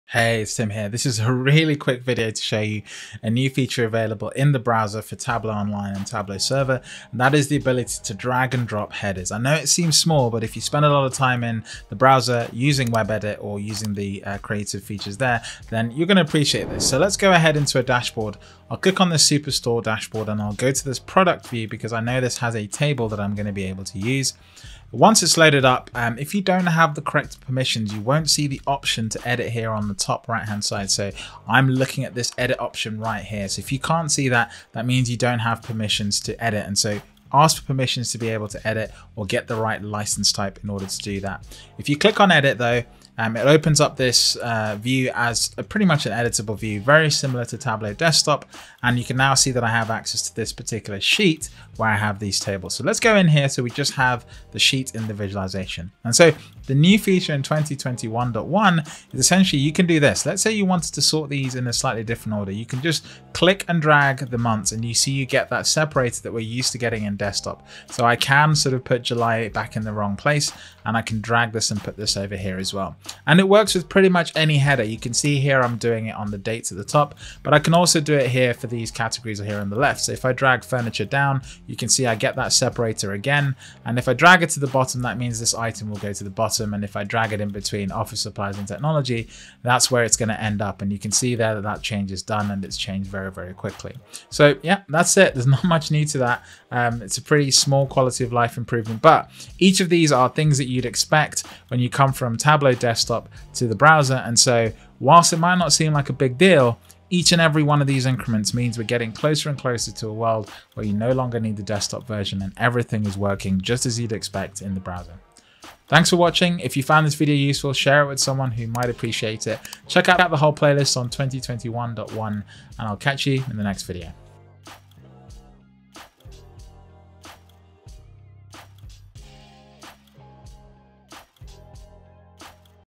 Drag and drop headers: New in Tableau 2021.1
Create a custom sort order by dragging-and-dropping headers to actively reorder columns/rows directly within a view on the web, just like in Tableau Desktop. Share feedback and Suggestions